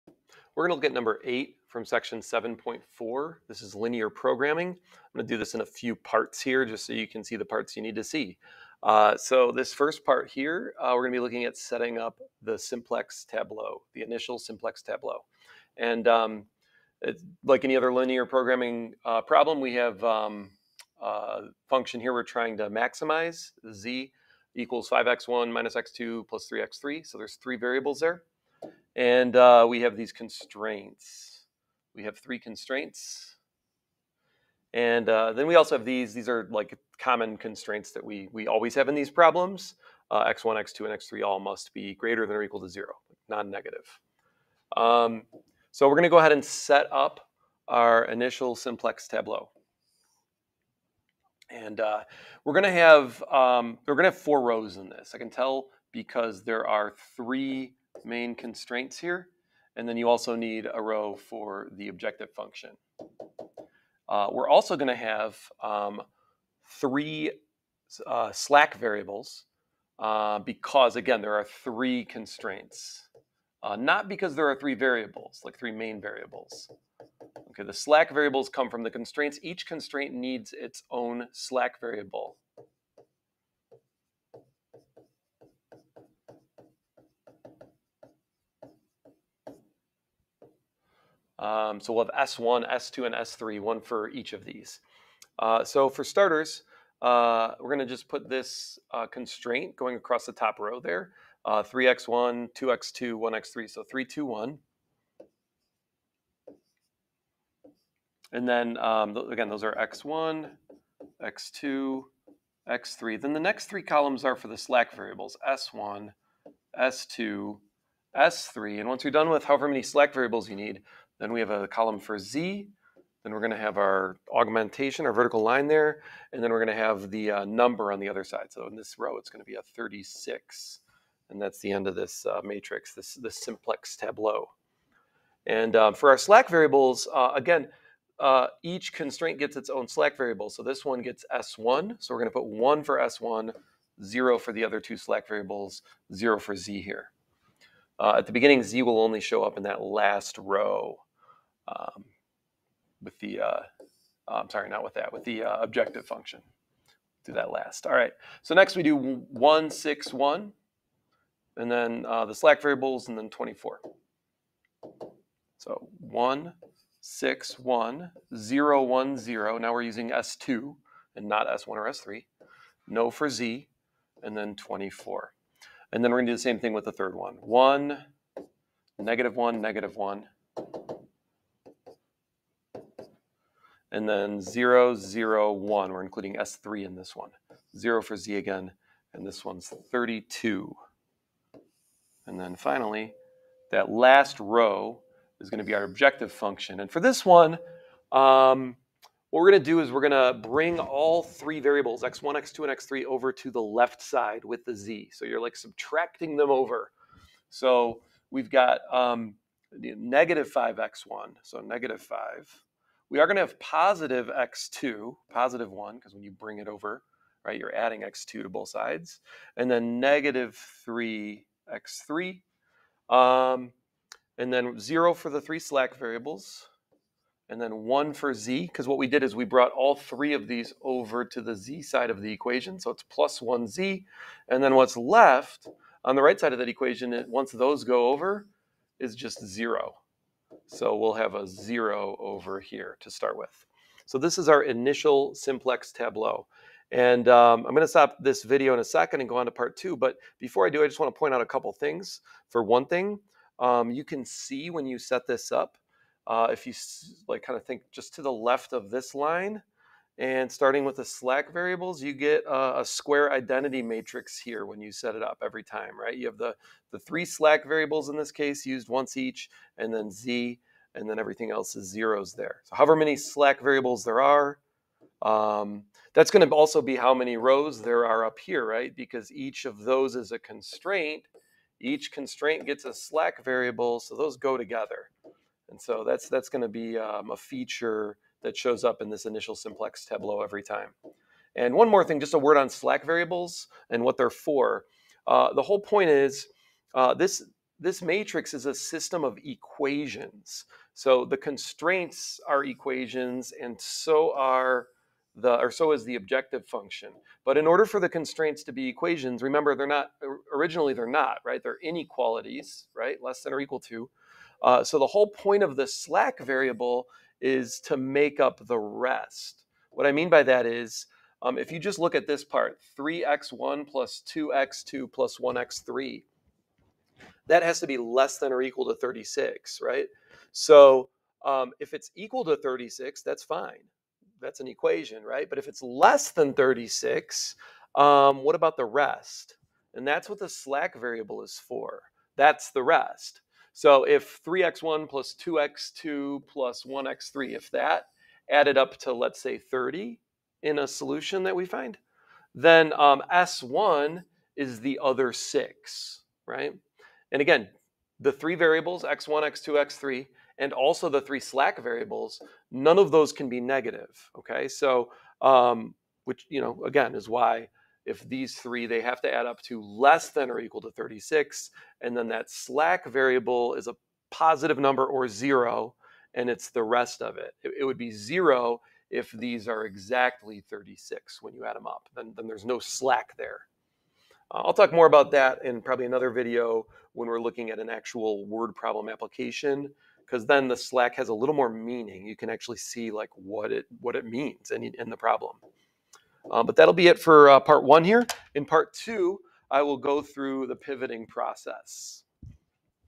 Linear Programming - 7.4 #8 part 1 - Simplex Method - Set Up Simplex Tableau
Video 1 of 3 on this example - going through the process of setting up the initial simplex tableau for the simplex method.

In video 2, I go through the pivoting process, and in video 3, I go through the interpretation.